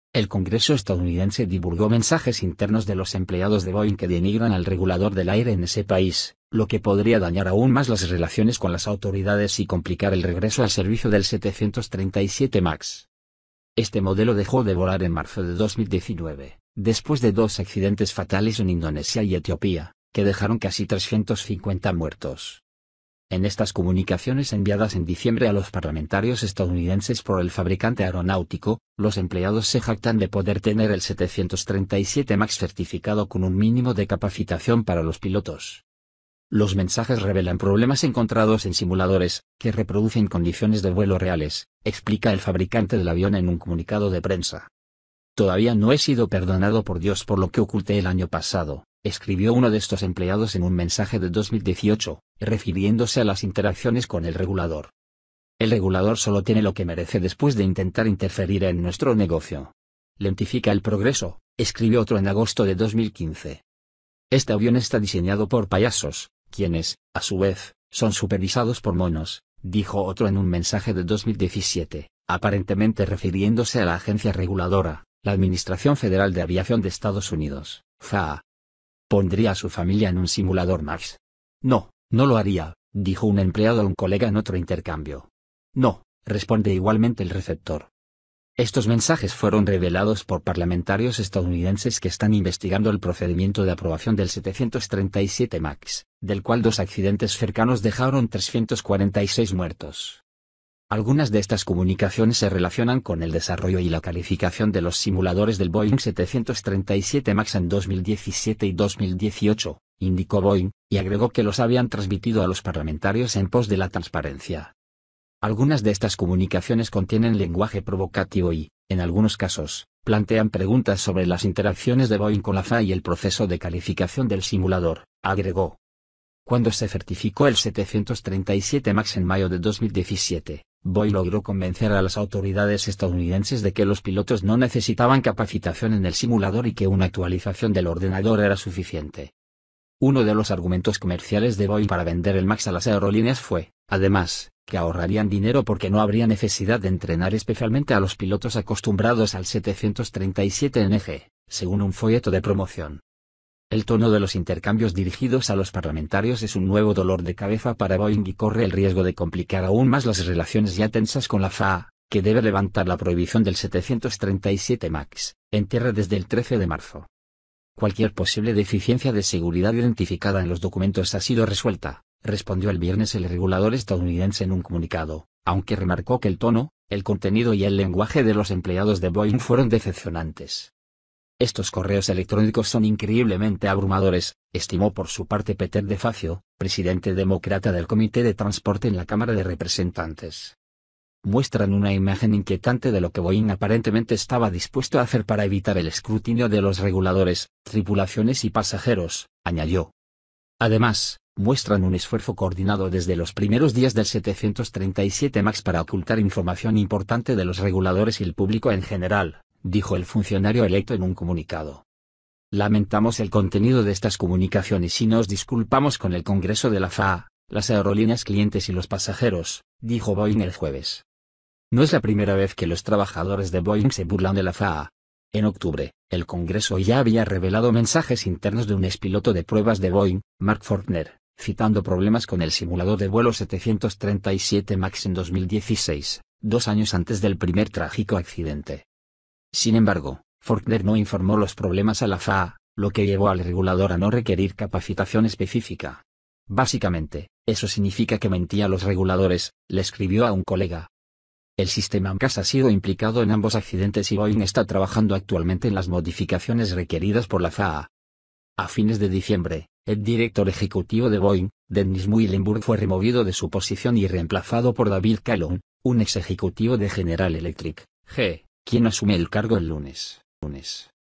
Escándalo en Boeing por mensajes de sus empleados
El Congreso estadounidense divulgó mensajes internos de los empleados de Boeing que denigran al regulador del aire en ese país, lo que podría dañar aún más las relaciones con las autoridades y complicar el regreso al servicio del 737 MAX.

Este modelo dejó de volar en marzo de 2019, después de dos accidentes fatales en Indonesia y Etiopía, que dejaron casi 350 muertos.

En estas comunicaciones enviadas en diciembre a los parlamentarios estadounidenses por el fabricante aeronáutico, los empleados se jactan de poder tener el 737 MAX certificado con un mínimo de capacitación para los pilotos.

Los mensajes revelan problemas encontrados en simuladores, que reproducen condiciones de vuelo reales, explica el fabricante del avión en un comunicado de prensa.